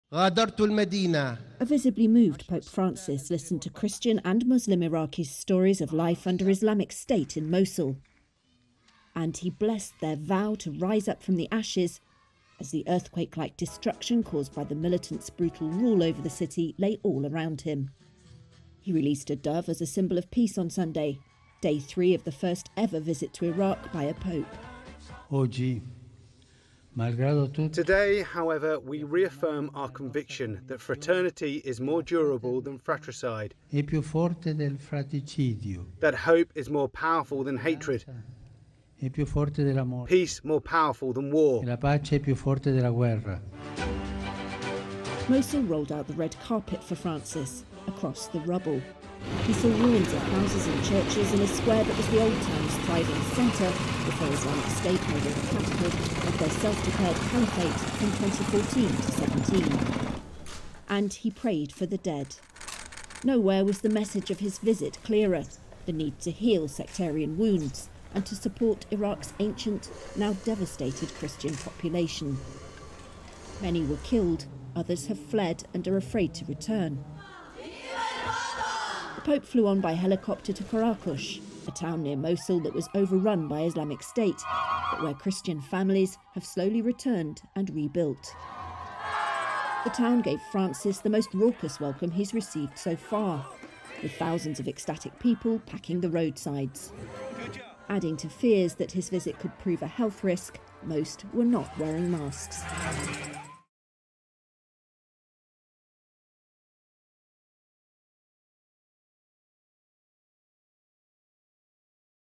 In Mosul, Pope Francis espouses peace, prays for the departed
Pope Francis prayed for peace, and for the dead, in the Iraqi city of Mosul on Sunday, before heading to the town of Qaraqosh, where Christian communities are starting to rebuild. Lucy Fielder reports.